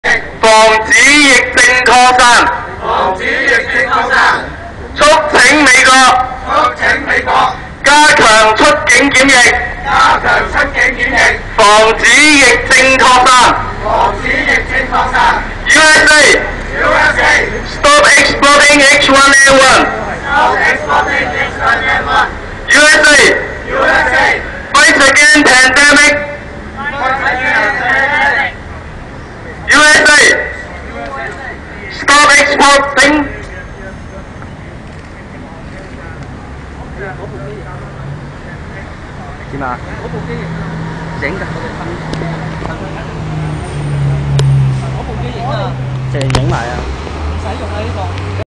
袁志光往美國領事館促請美國政府加強旅客出境檢疫措施, 共抗豬流感
工聯會社區幹事袁志光於09年5月18日下午往美國領事館促請美國政府加強旅客出境檢疫措施, 共抗豬流感.